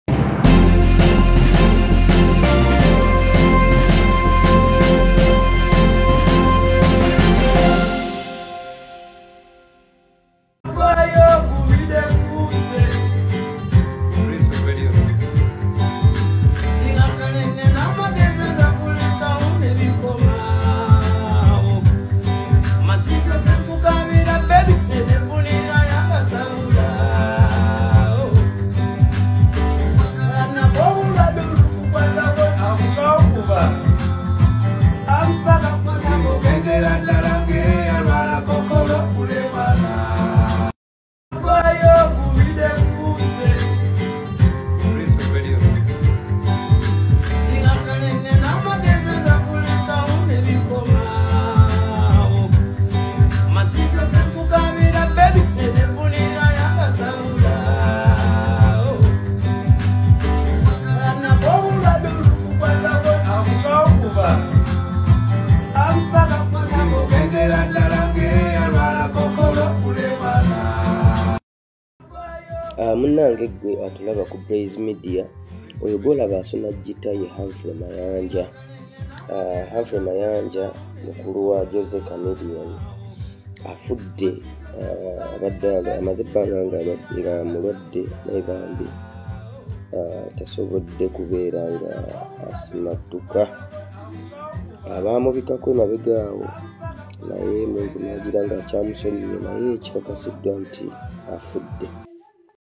Breaking News!! Humphrey Mayanja, 's Last Video Before his Death with Jose Chameleon
Humphrey Mayanja's last Video with Jose Chameleone